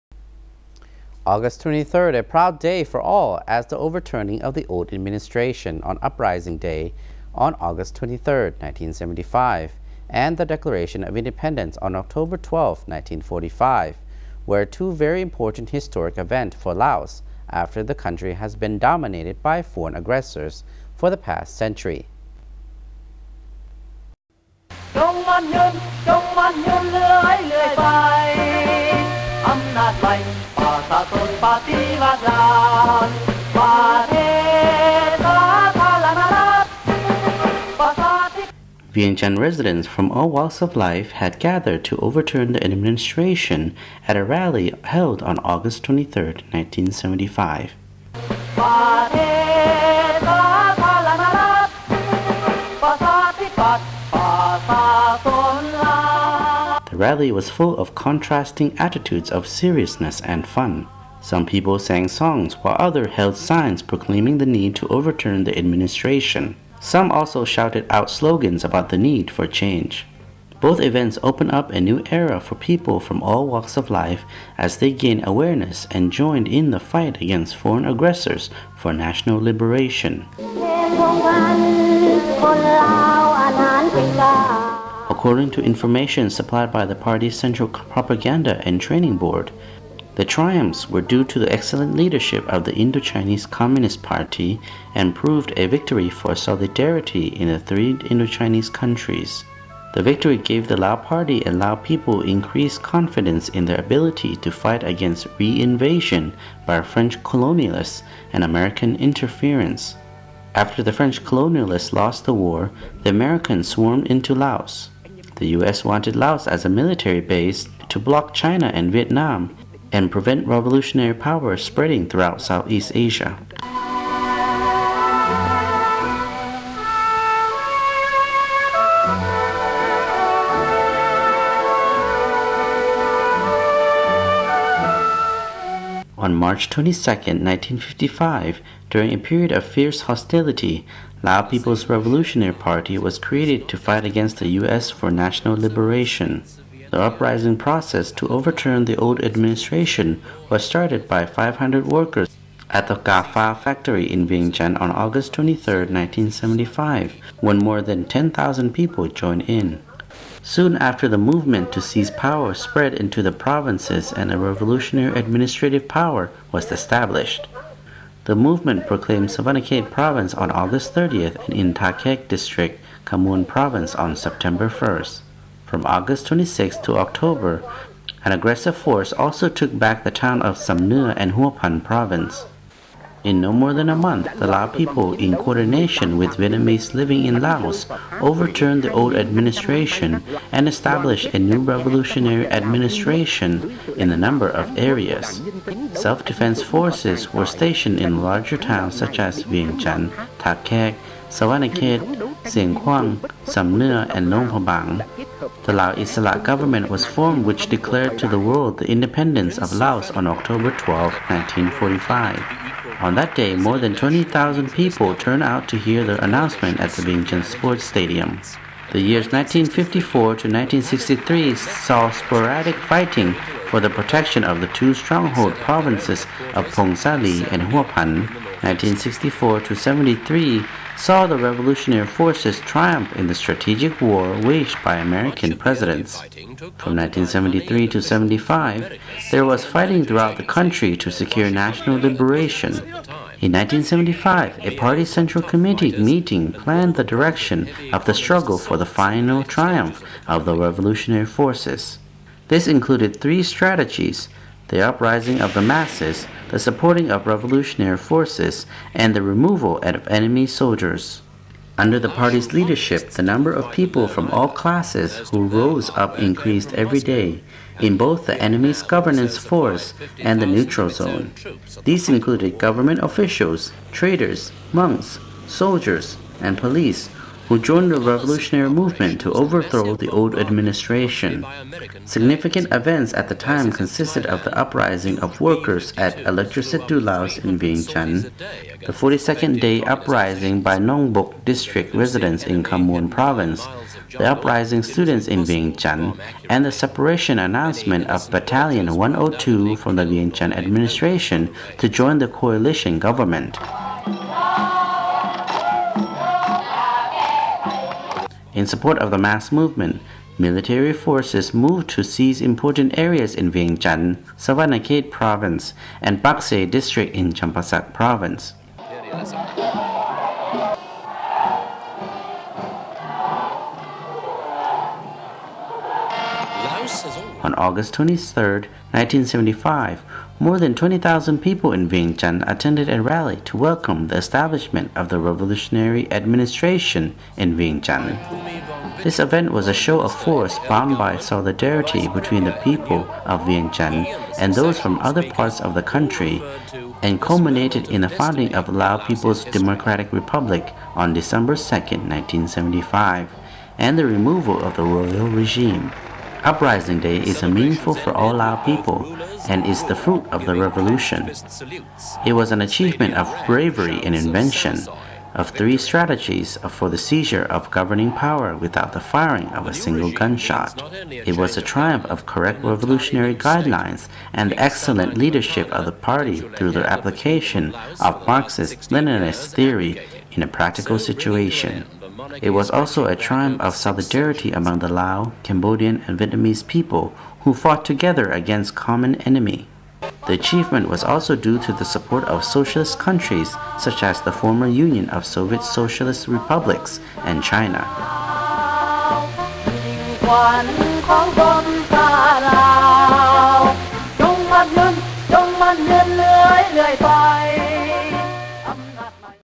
Lao NEWS on LNTV: August 23, a day for overturning of the old administration.23/8/2016
VO August 23, a proud day for all as the overturning of the old administration
INTRO: August 23, a proud day for all as the overturning of the old administration on Uprising Day on August 23, 1975, and the declaration of independence on October 12, 1945, were two very important historic events for Laos after the country had been dominated by foreign aggressors for the past century.